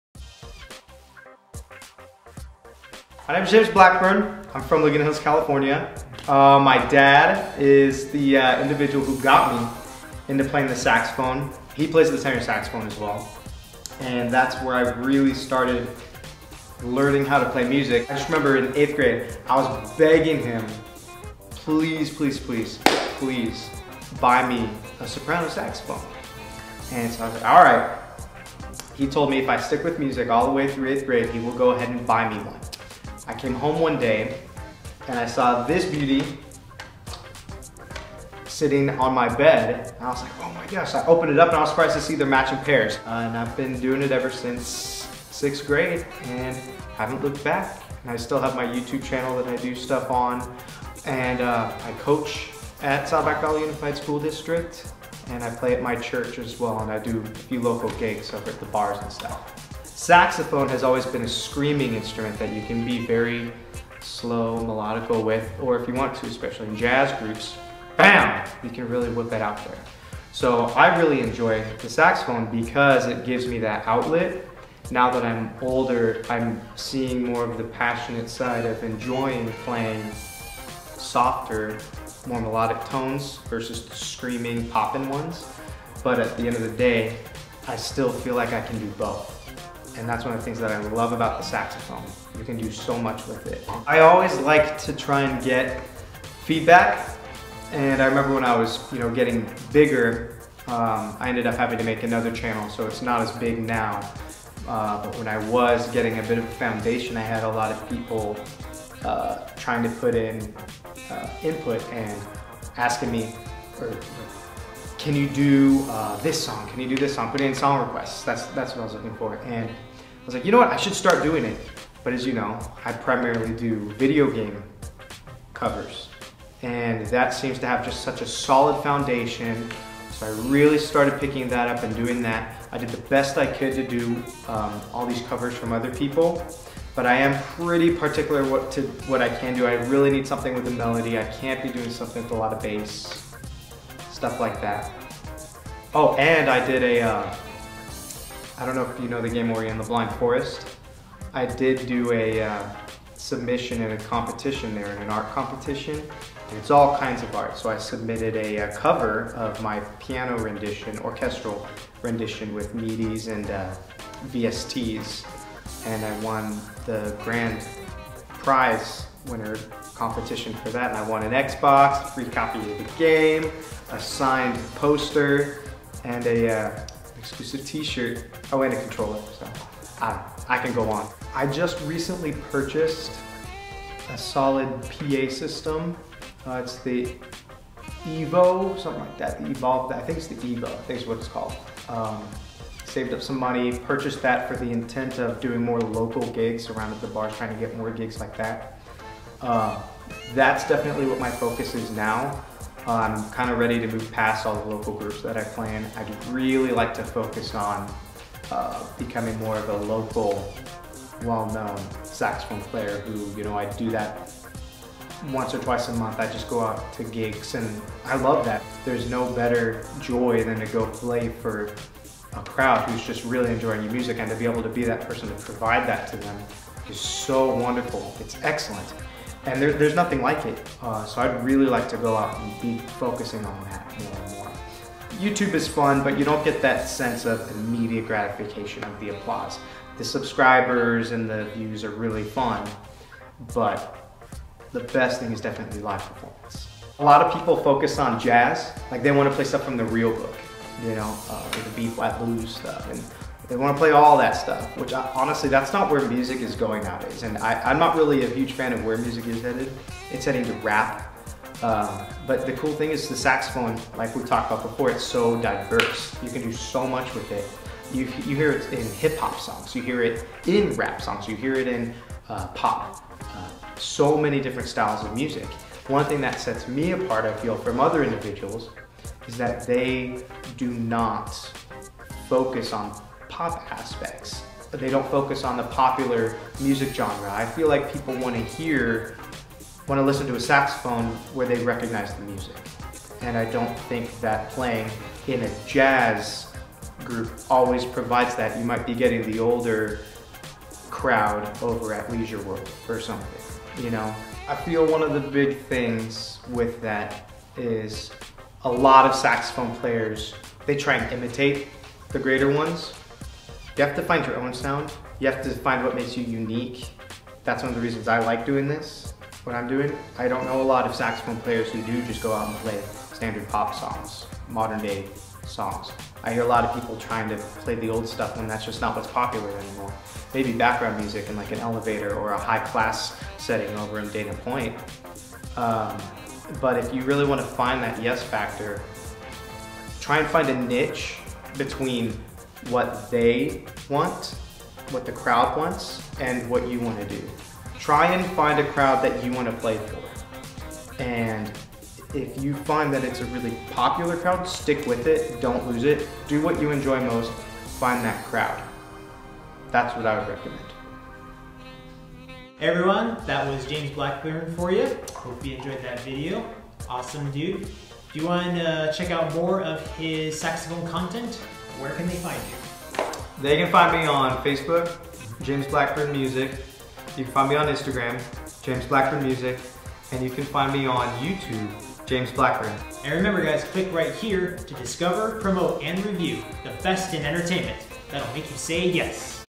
James Blackburn | Musician PowWow | YES
James Blackburn is a saxophone player from Southern California.  He covers songs, primarily video game songs and anthems.  He is also very active in performing and playing for as many gigs as he can around his community.  This is a PowWow looking into what a musician aspires to do within his or her community.  Check out his projects and his style.